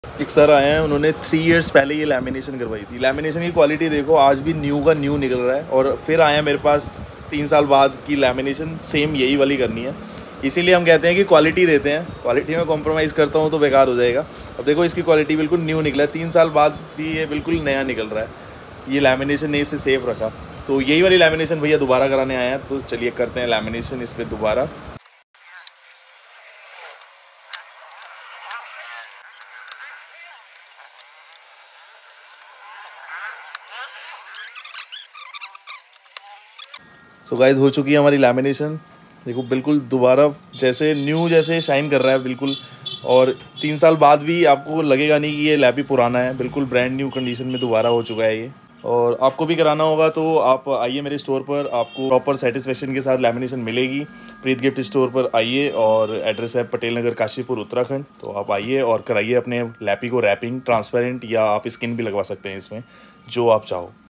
Dell Laptop lamination again after 3 years|| 100% satisfied customer || preet gift store
Dell Laptop lamination again after 3 years and customer was full satified with our top quality lamination
Full hight quality lamination glossy and matte available
And also all type of colouful lamination available at our store for phones and laptops
You can also protect you loved phones and laptops from future scratches
Address: preet gift store
Patel nagar kashipur
Uttarakhand
244713